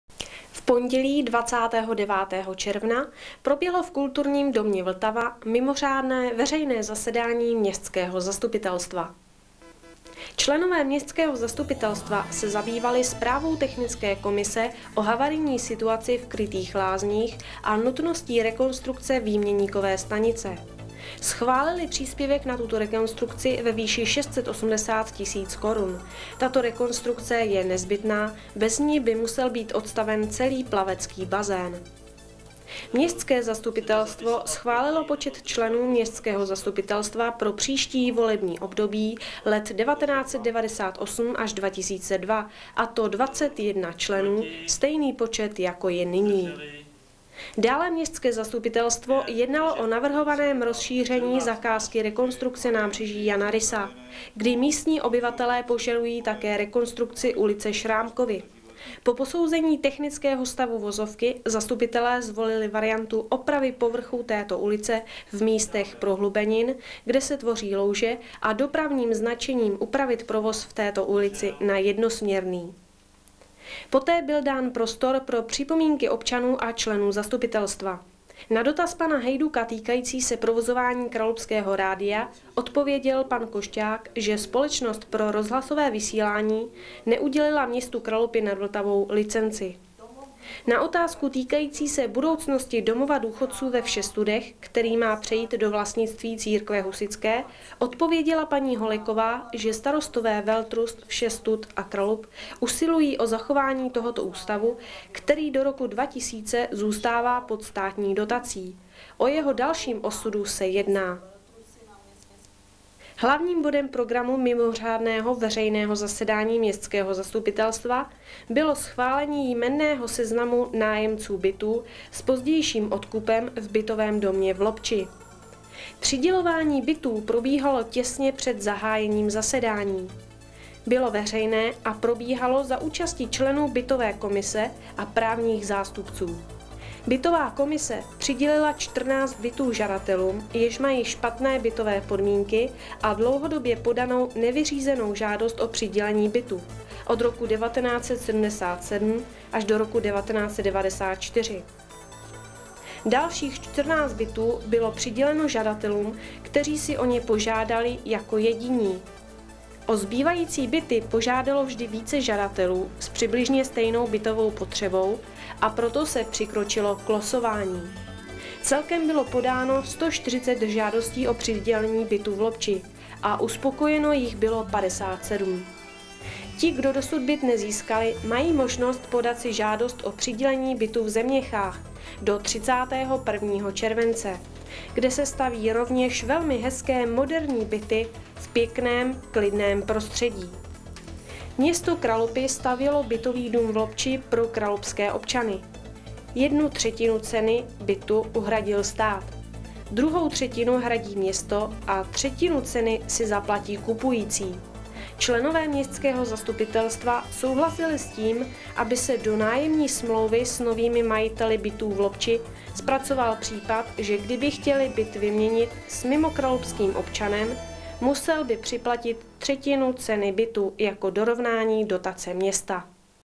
Kralupy TV: Mimořádné zasedání Zastupitelstva města Kralupy nad Vltavou (4. 7. 1998)
Mimořádné zasedání Zastupitelstva města Kralupy nad Vltavou.